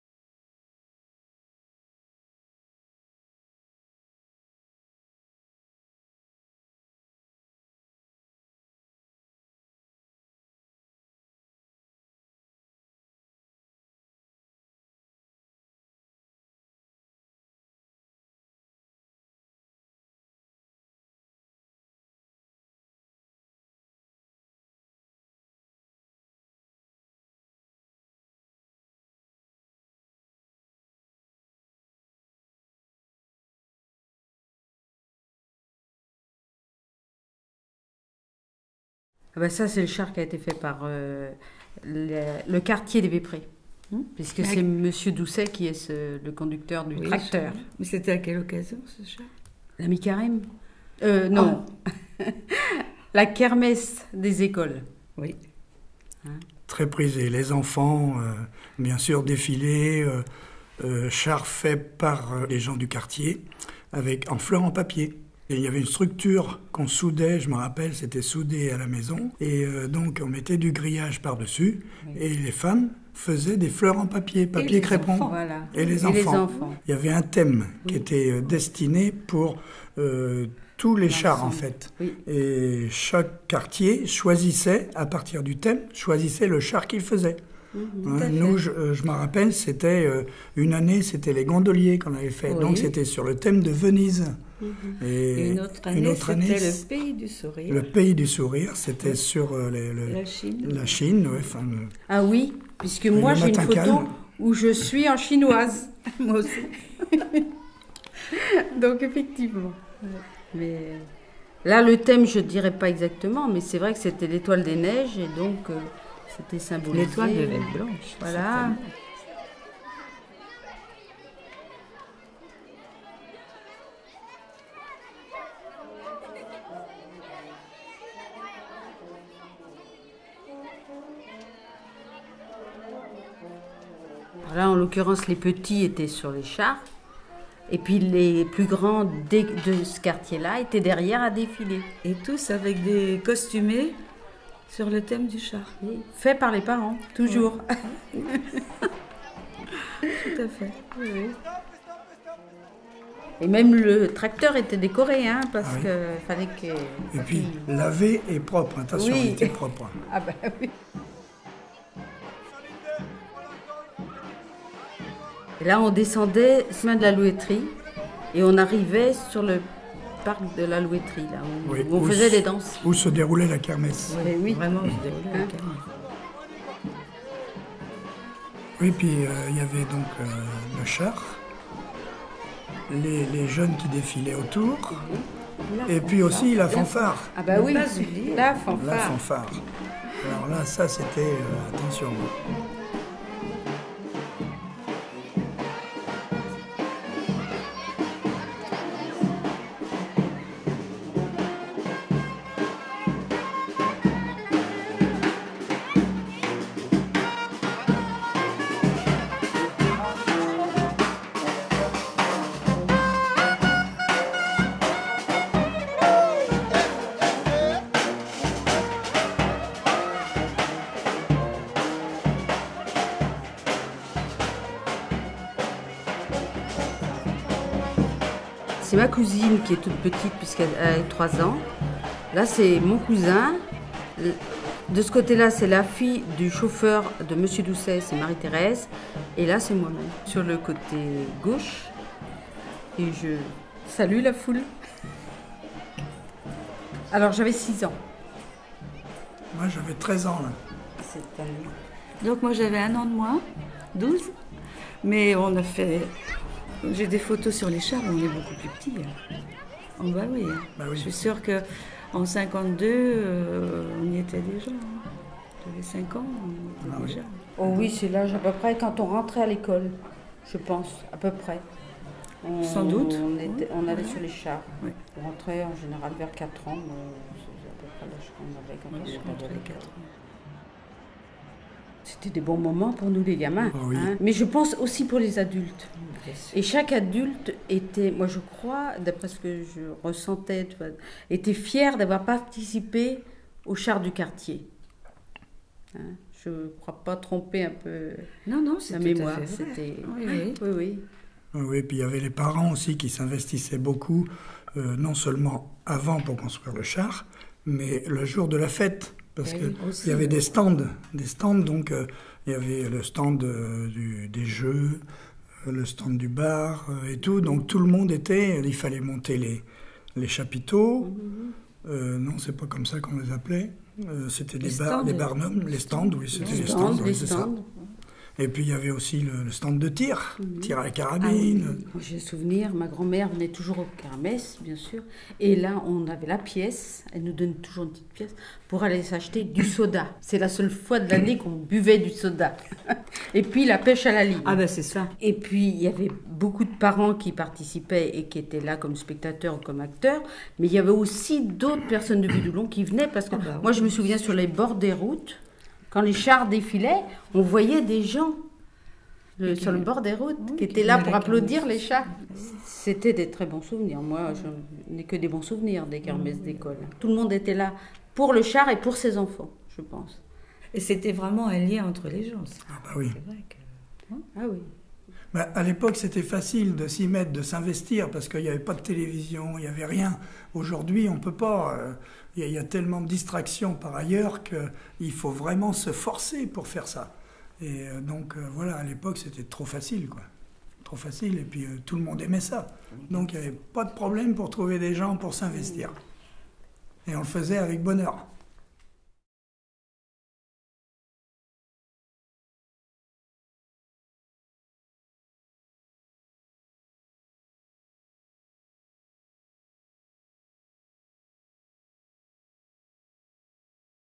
Images sonores du Vieux-Doulon : jour de fête (6/7)
Mesdames Paulette Cassard et Thérèse Soulard et monsieur Michel Chassé se remémorent la kermesse de l'école, la fanfare et les chars fleuris au papier crépon... C'est jour de fête au quartier du Vieux-Doulon de Nantes.